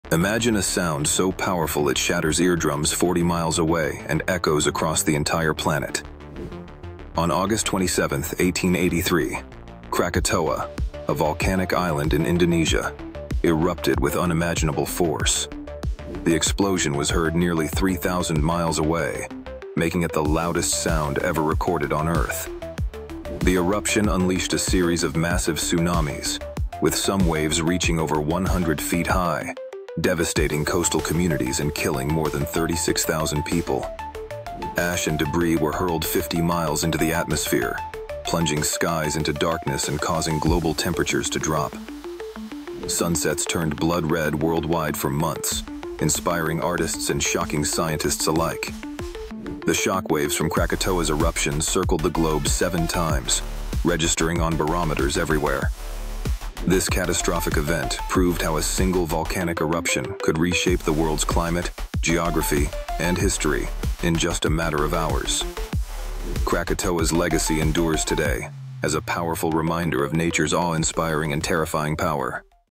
The Volcano That Shook the Whole World!
Discover how the 1883 Krakatoa eruption unleashed the loudest sound ever recorded and changed the world forever.